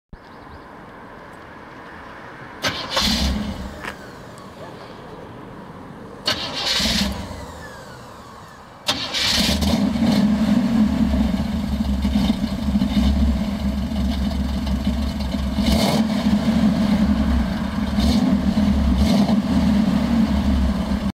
67`Chevrolet Camaro Startup Sound !
That is what you can call the `show off effect`... but it runs very good.